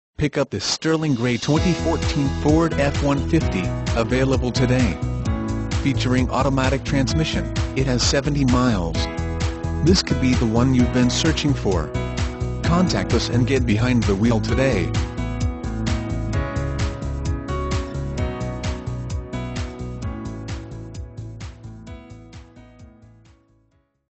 2014 FORD F-150 Macon, GA 14T701
2014 FORD F-150 Macon, GA
Stock# 14T701

Pick up this Sterling Gray 2014 Ford F-150, available today. Featuring Automatic transmission, it has 70 miles. This could be the one you've been searching for.